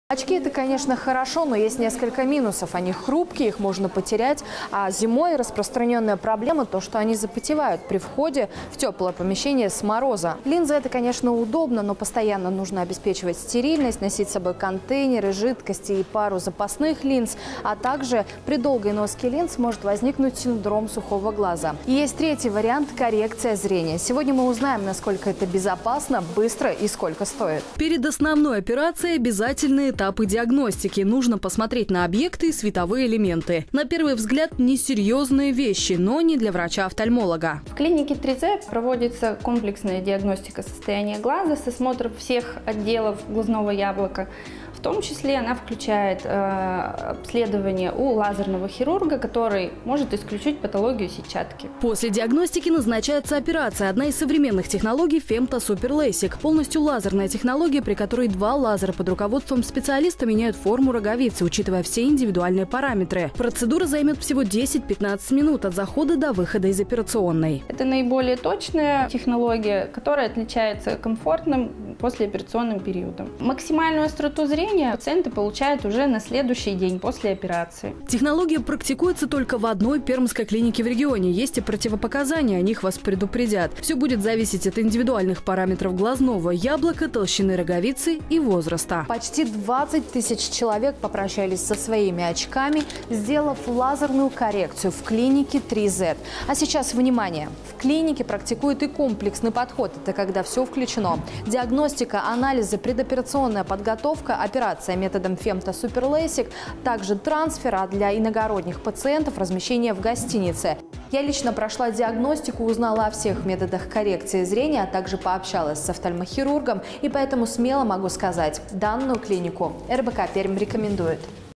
РБК Пермь рекомендует: 3Z
Очки — дома, линзы — на работе, плохое зрение — уже как привычка. Если вы узнали себя в этих трёх пунктах — возможно, пора задуматься о лазерной коррекции зрения*.

А РБК Пермь уже подобрал для вас подходящую клинику — мы рекомендуем 3Z**

*имеются противопоказания, необходима консультация специалиста
**ООО «Три-З». Лицензия №Л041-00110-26/00589165 от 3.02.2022